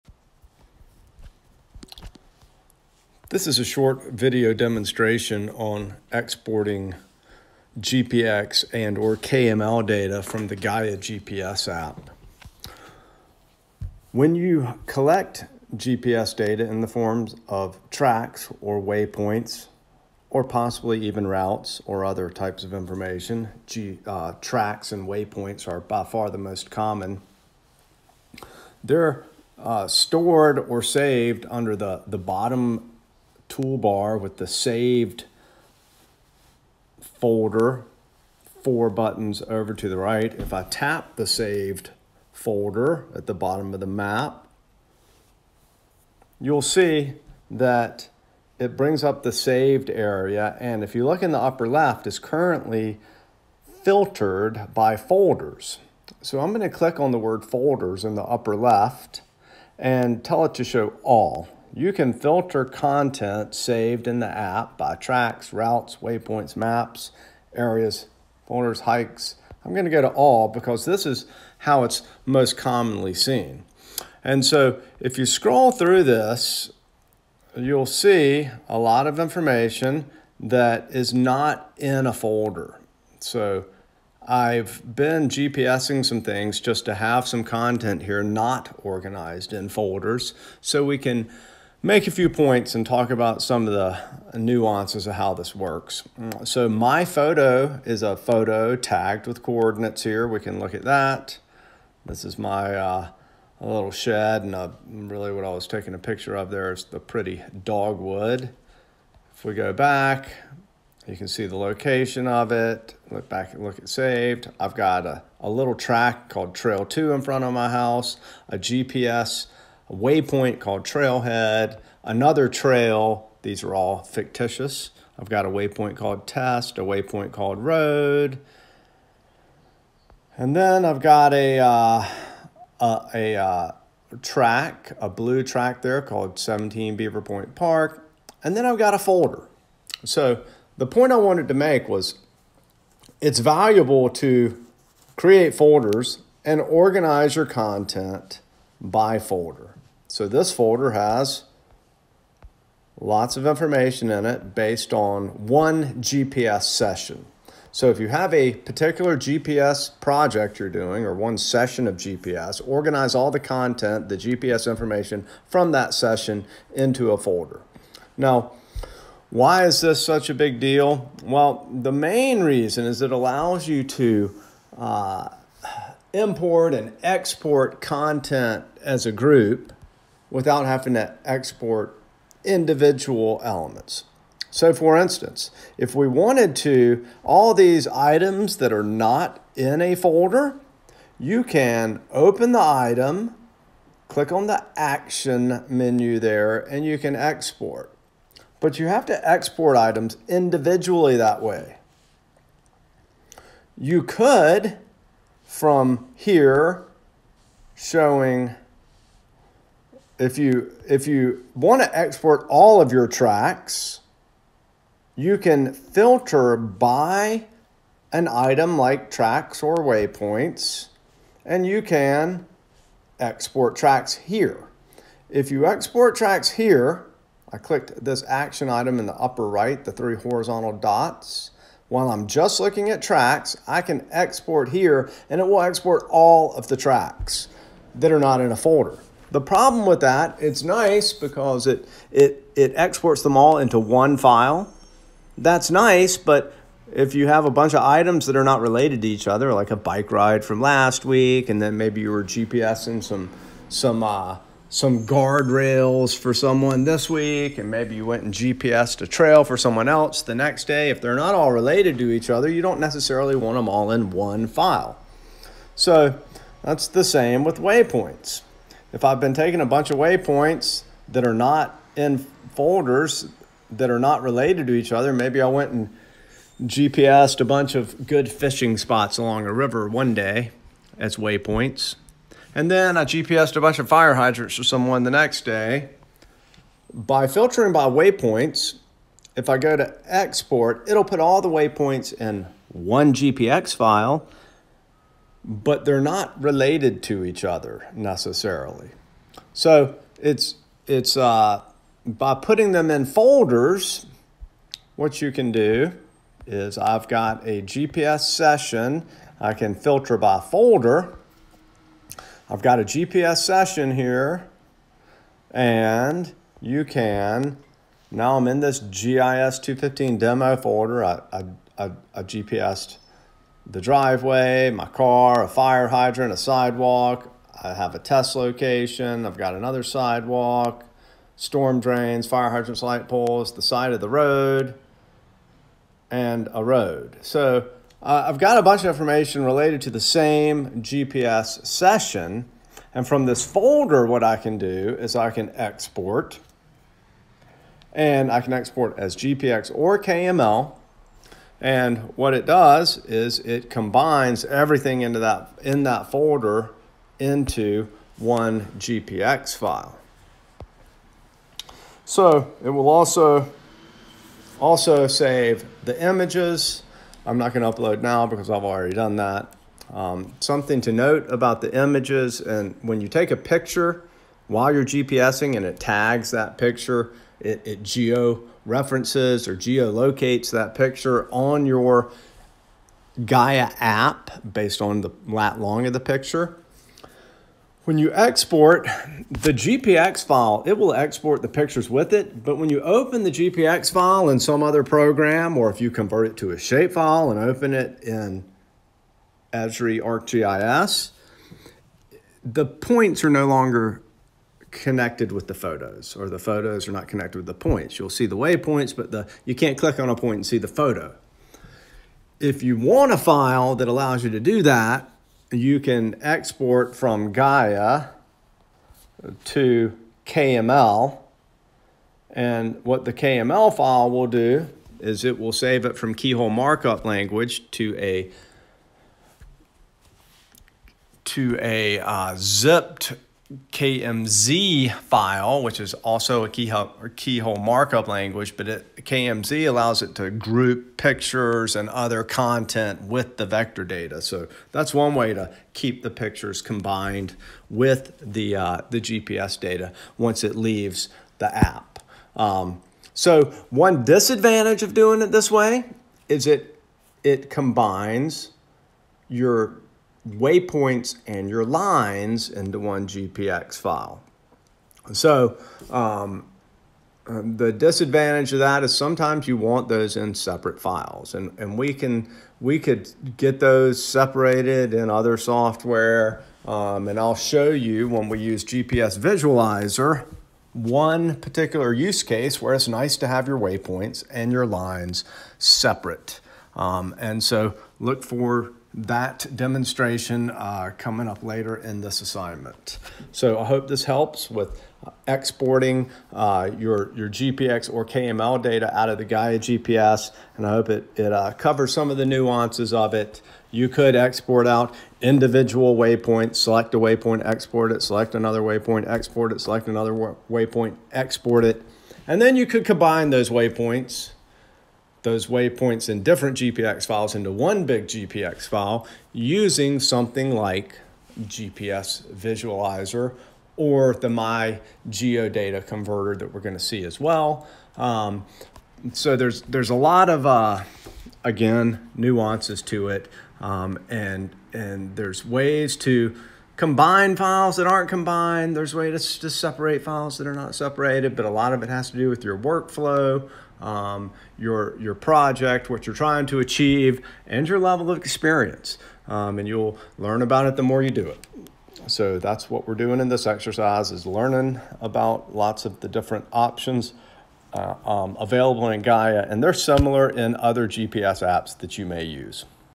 Gaia GPS - Export GPS Data
Export GPS data (tracks and waypoints) to GPX and KML.